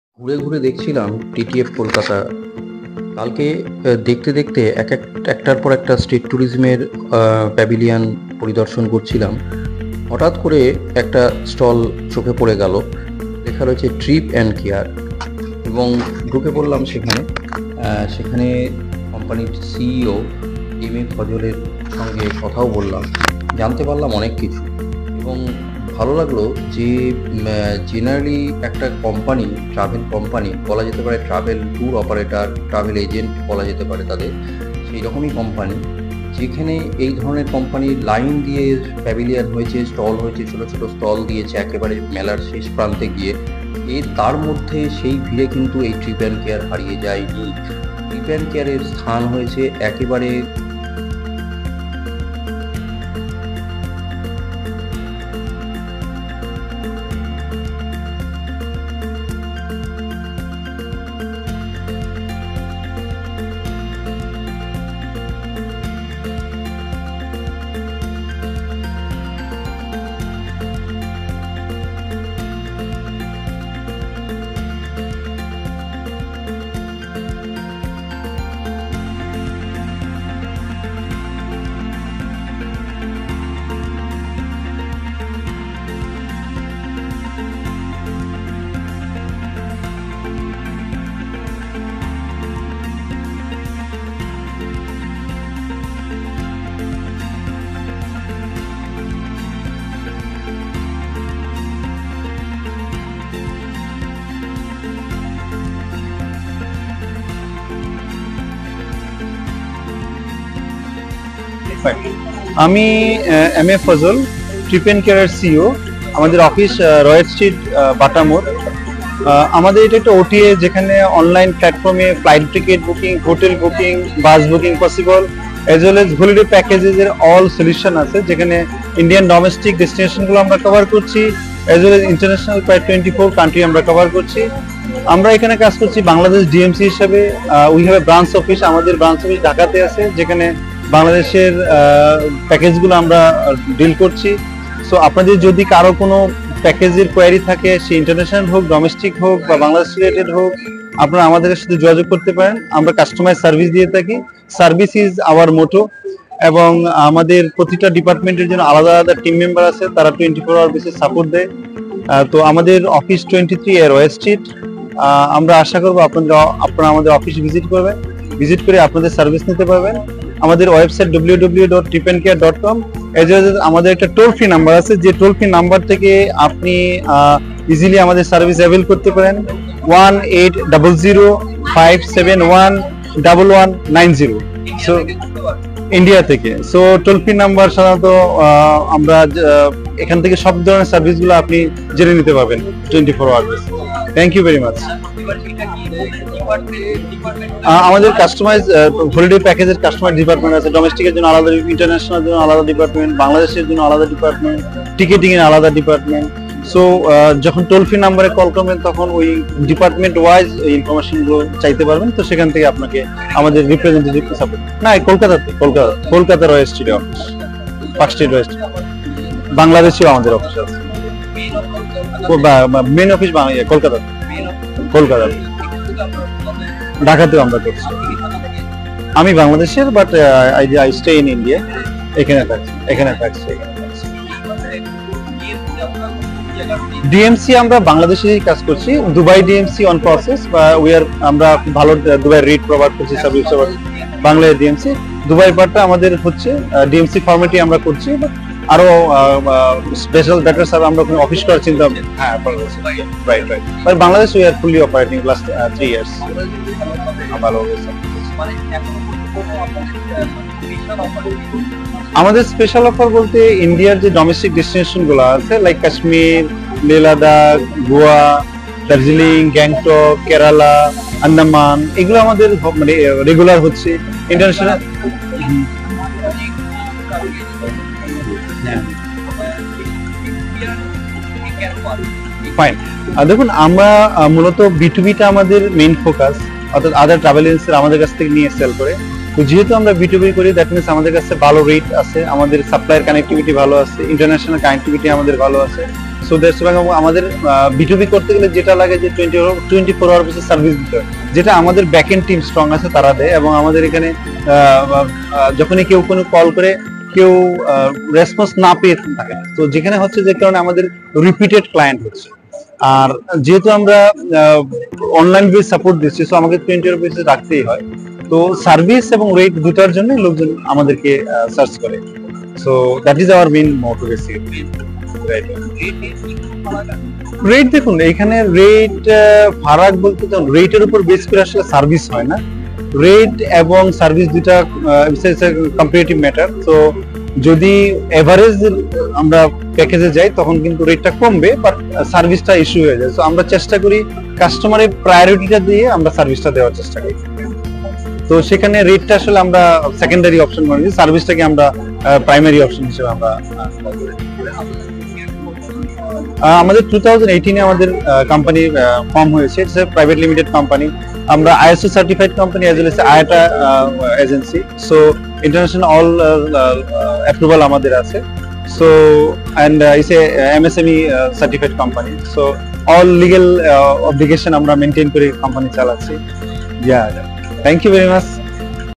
TTF KOLKATA : ভ্রমণপ্রেমীদের নজর কেন Tripncare ? জেনে নিন
TTF KOLKATA এবারও নানা ধরনের ভ্রমণের প্যাভেলিয়ন হয়েছে। তার মধ্যে যে সমস্ত ট্রাভেল কোম্পানি নজর কেড়েছে তাদের মধ্যে উল্লেখযোগ্য TRIPNCARE। কেন তা জানতেই হাজির হয়েছিলাম তাদের স্টলে। সেকথাই জানালেন কোম্পানির CEO M A Fazal.